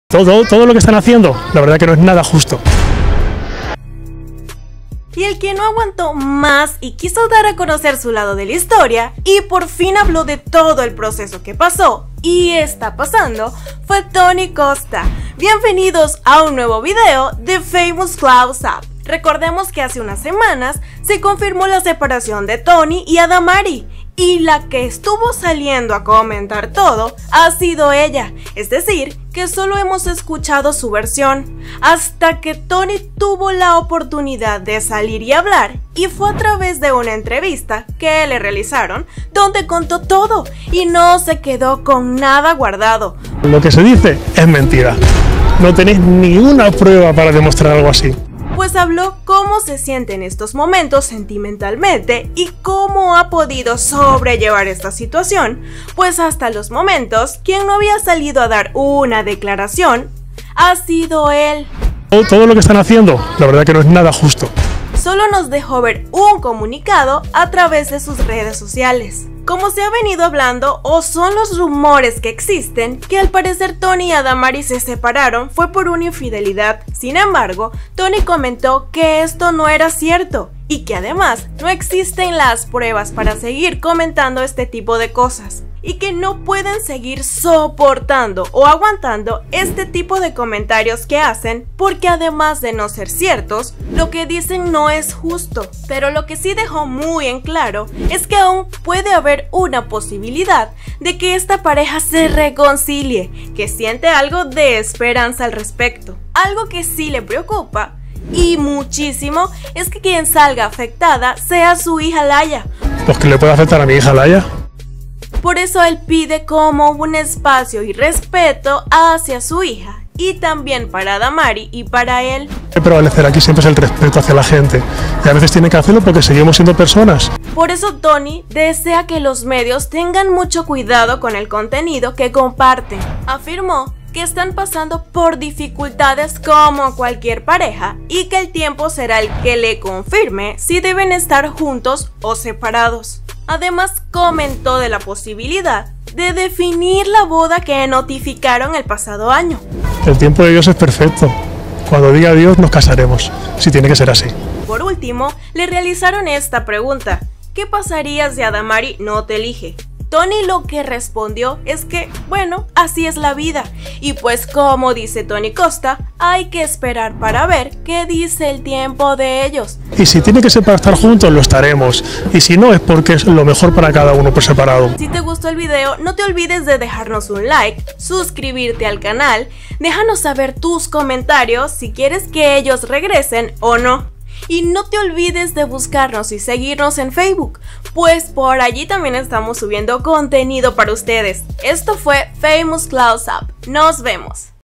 🛑📌 𝗨𝗟𝗧𝗜𝗠𝗔 𝗛𝗢𝗥𝗔 𝗗𝗔 𝗟𝗔 𝗖𝗔𝗥𝗔 Toni Costa a Javier Ceriani 😱
Toni Costa revela toda la verdad sobre la infidelidad